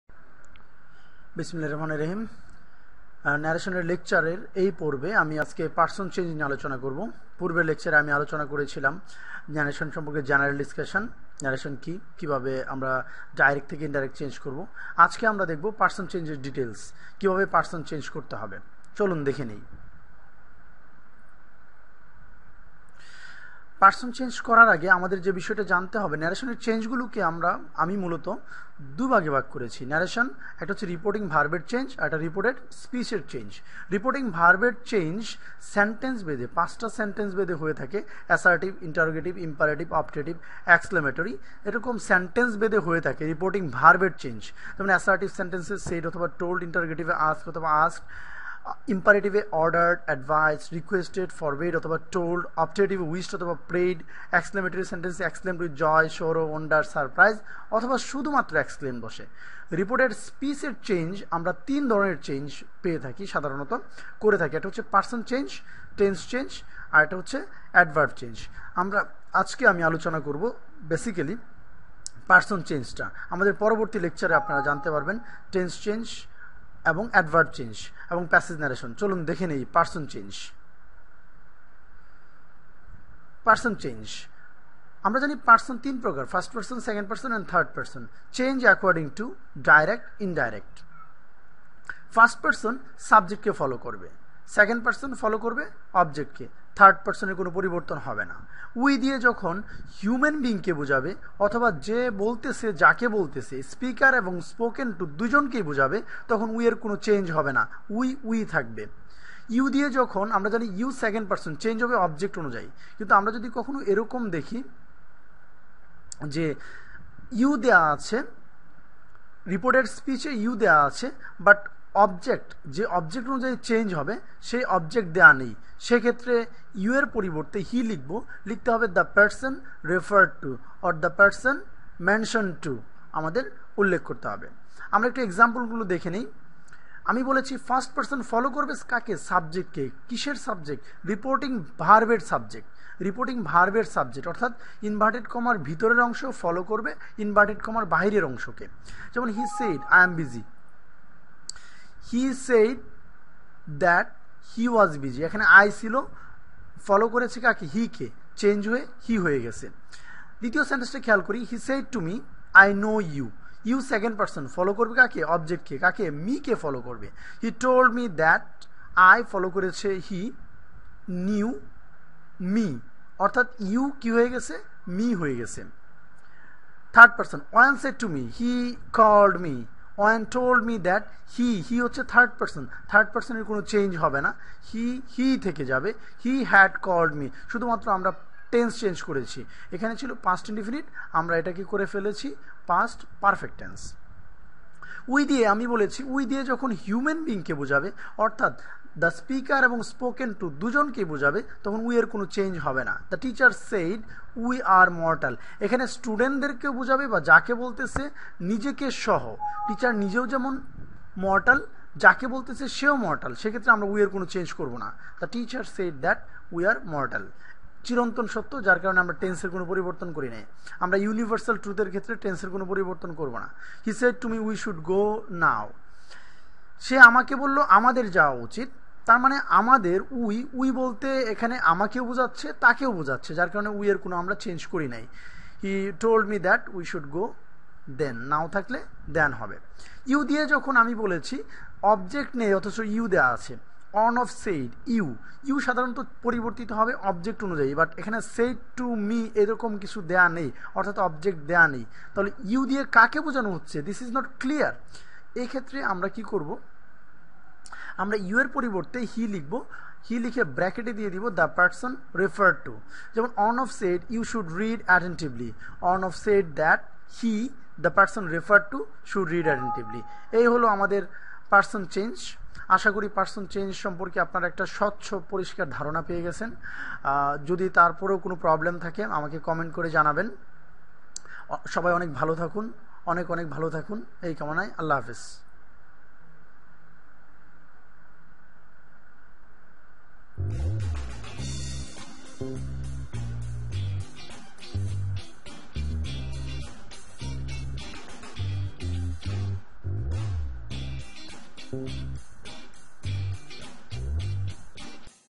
Narration -Direct to Narration Person Change
Person change of direct to indirect is available here in the lecture. The lecture is in Bengali language. If you have any suggestion regarding the lecture please feel free to write me through comment or in facebook fan